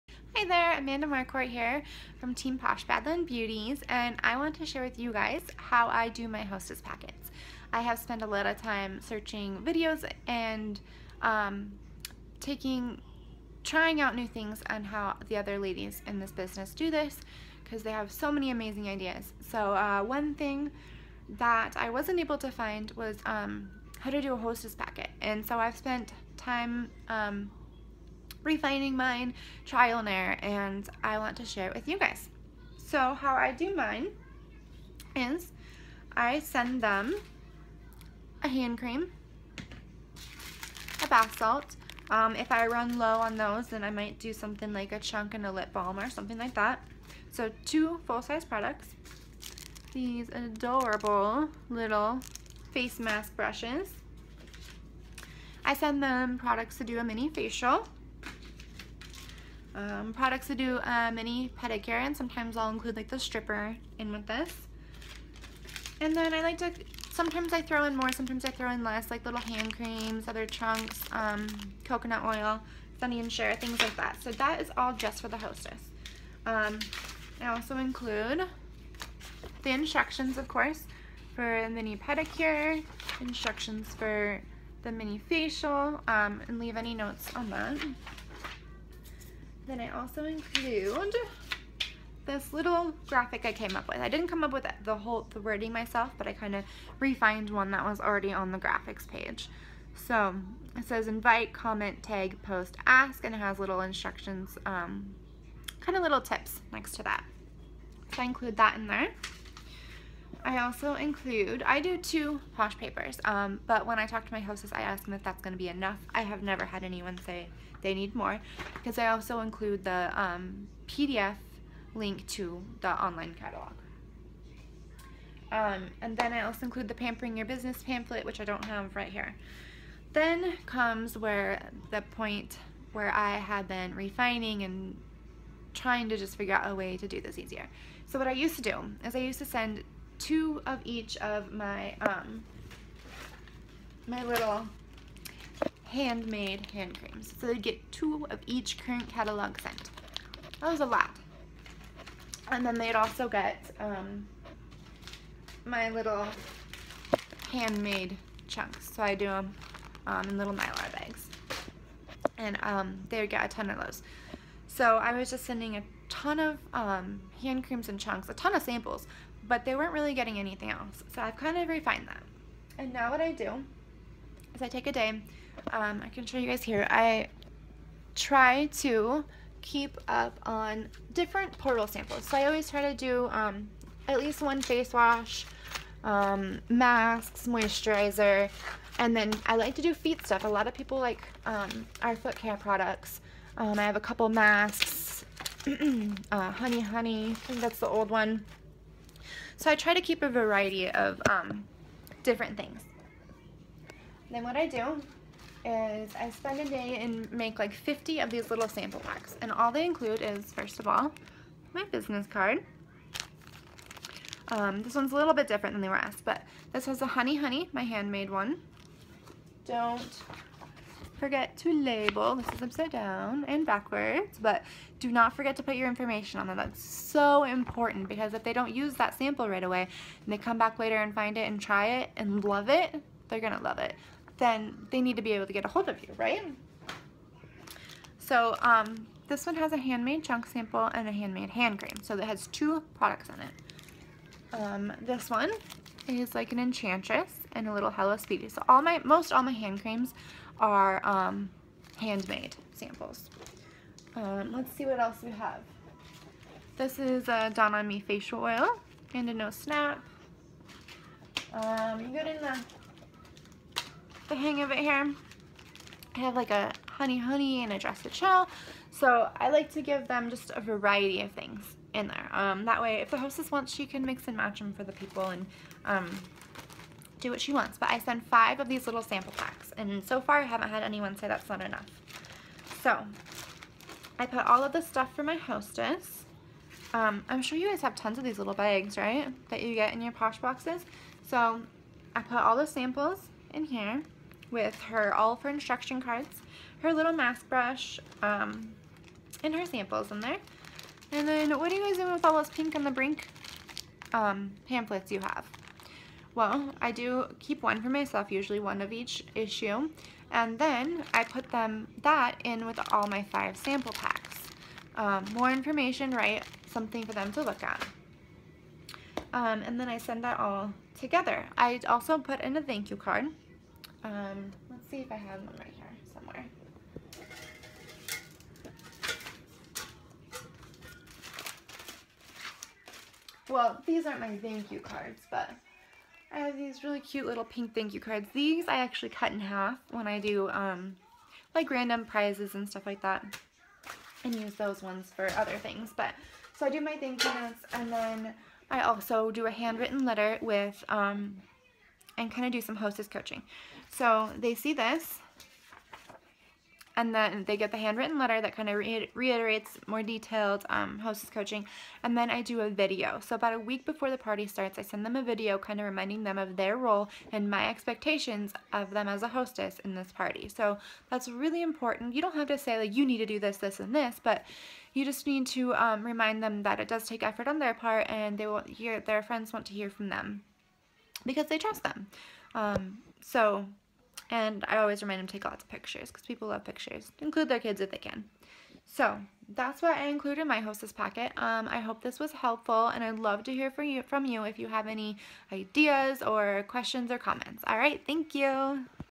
How to Hostess Packet
There are tons of ways to Pamper your Hostess. Here's what I include in mine. Even if this way doesn't work for you I hope it inspires you to find your own system.